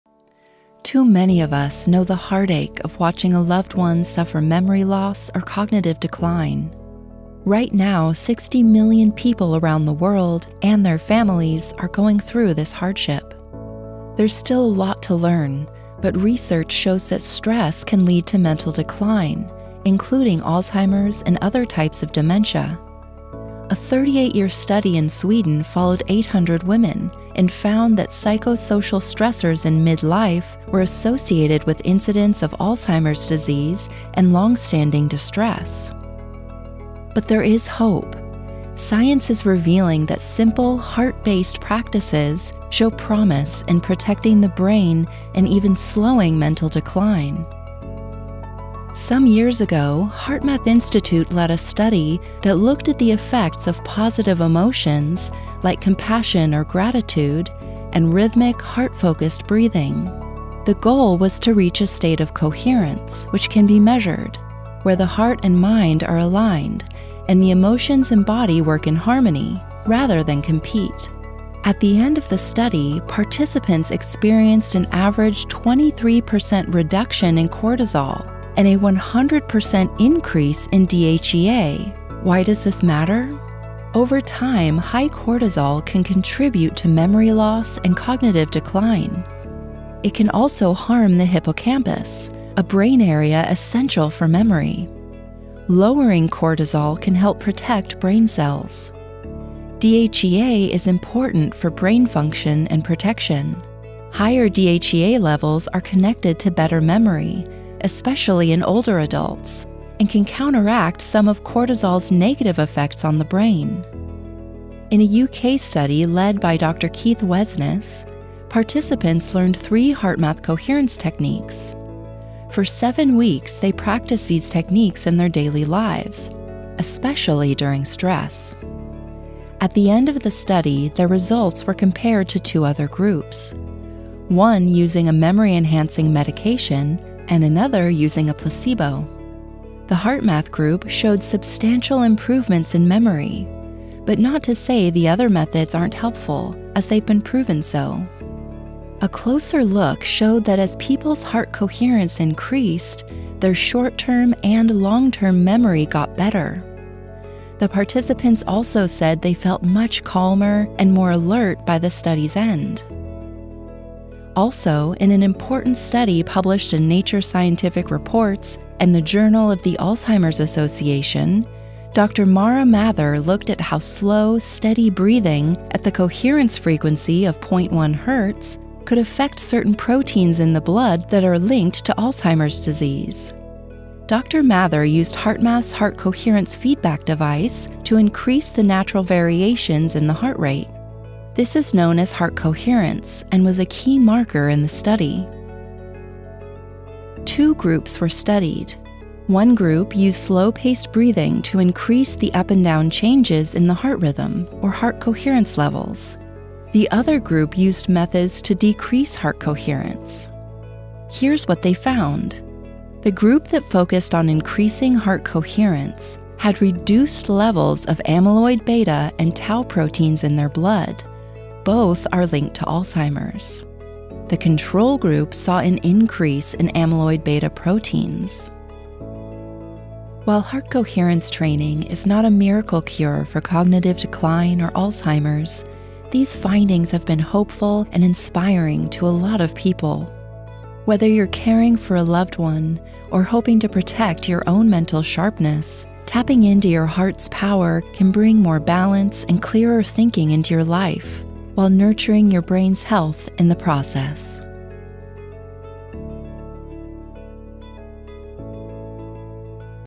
Heart Coherence and Memory: Science-Backed Hope for Helping Mental Decline
Discover a groundbreaking approach to protecting your brain and memory as you age. In this video, we explore the fascinating science behind heart coherence, and how simple, heart-focused practices are showing incredible promise in slowing cognitive decline, including Alzheimer’s and dementia.

Backed by research from HeartMath Institute and other leading studies, you’ll learn how aligning your heart and mind can reduce harmful stress hormones, enhance memory, and even impact proteins linked to Alzheimer’s.

Whether you're caring for a loved one or seeking to safeguard your own mental sharpness, this video offers hope, practical tools, and a deeper understanding of the powerful connection between your heart and brain.

Research citations:

Swedish Midlife Stress Study:
Johansson L, et al. Midlife psychological stress and risk of dementia: a 38-year longitudinal population study. BMJ Open (2013).

Cortisol/DHEA and Coherence Training:
McCraty, R. (2016). Science of the Heart: Exploring the Role of the Heart in Human Performance (Volume 2). HeartMath Institute, Boulder Creek, CA.

UK Memory Improvement Study:
Wesnes KA, et al. HeartMath coherence training improves cognitive performance. Cognitive Drug Research Ltd. (2005).

Amyloid-Beta and Tau Protein Study:
Mather M, et al. Modulating heart rate oscillation affects plasma amyloid beta and tau levels. Scientific Reports (2023).